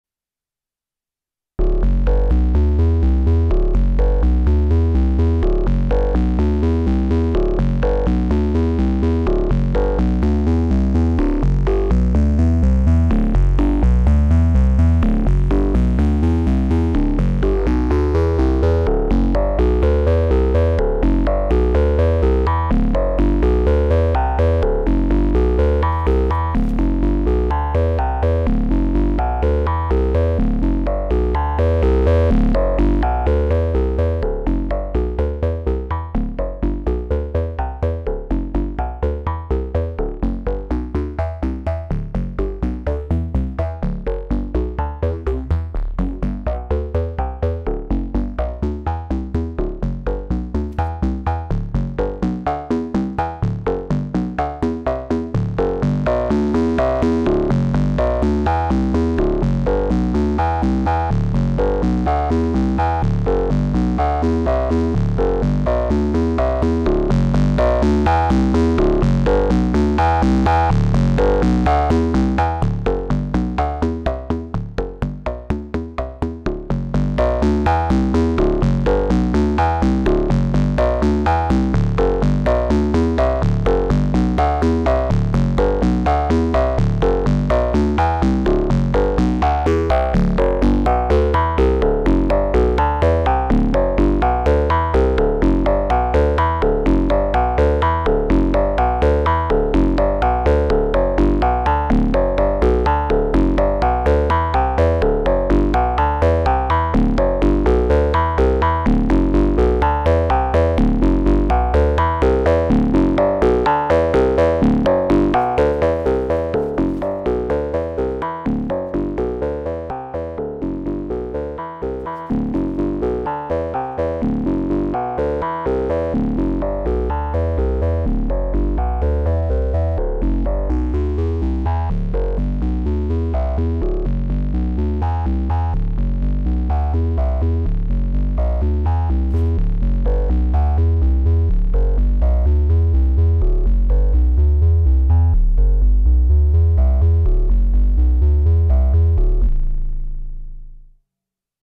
0-Coast minijam with SQ-1 and Cloudburst
Simple impromptu jamming over Make Noise 0-Coast knobs with the help of Korg SQ-1 double sequencing and a bit of Strymon Cloudburst reverb.